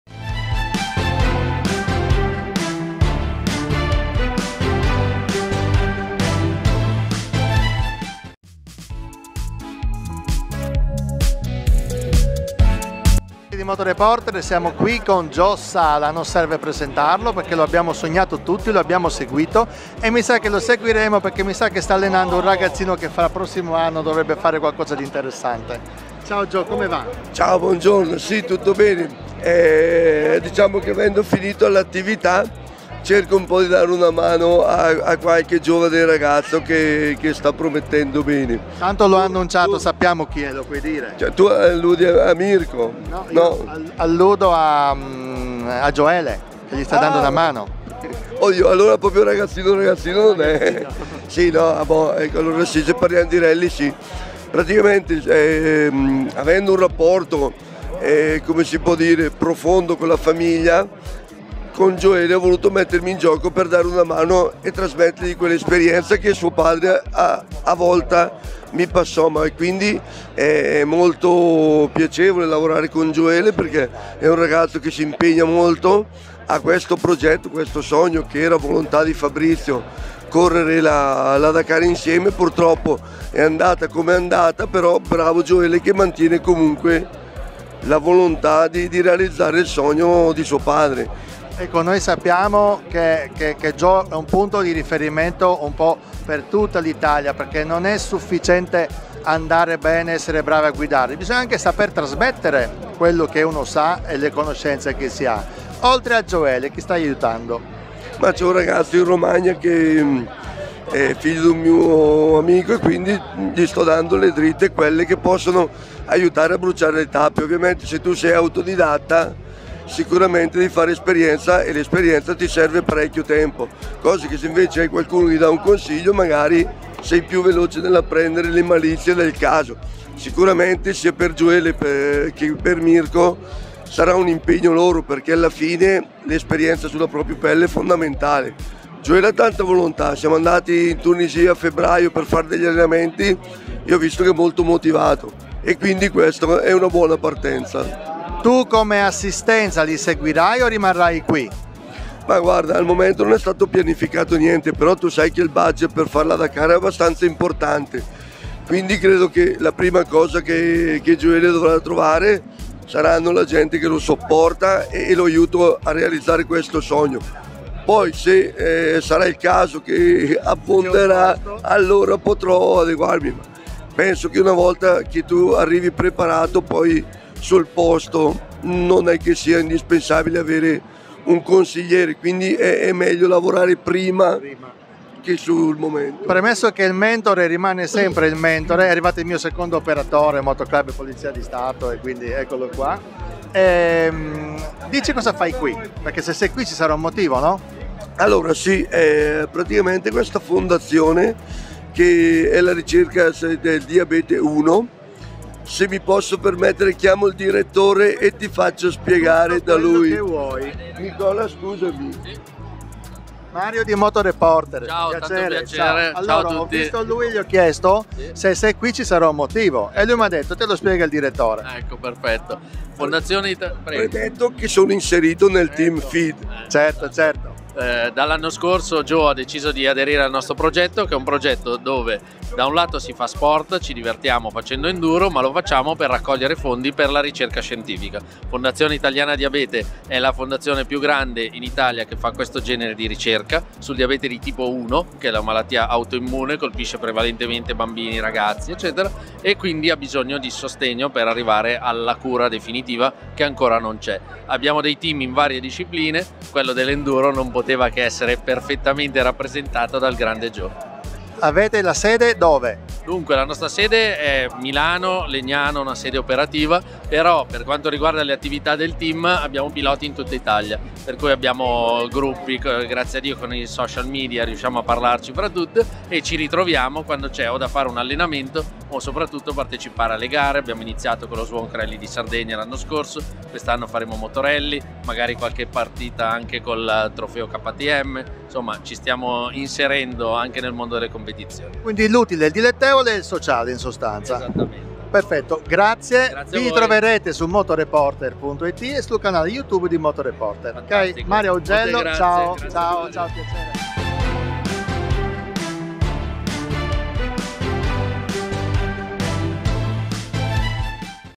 MBE 2023 Abbiamo incontrato Giò SALA
Alla fiera d Verona abbiamo incontrato Giò SALA, non servono presentazioni.
Ci siamo fatti raccontare cosa sta facendo ascoltiamolo.